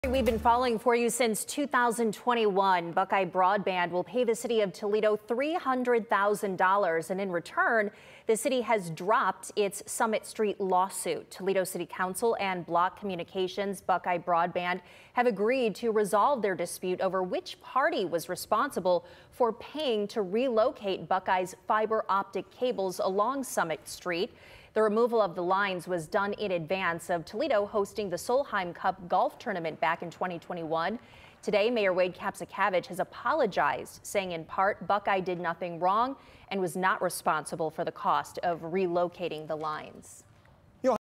Buckeye Broadband to pay $300,000 to resolve city lawsuit
The city of Toledo sued the cable company to recover costs for relocating cable lines during a Summit Street improvement project.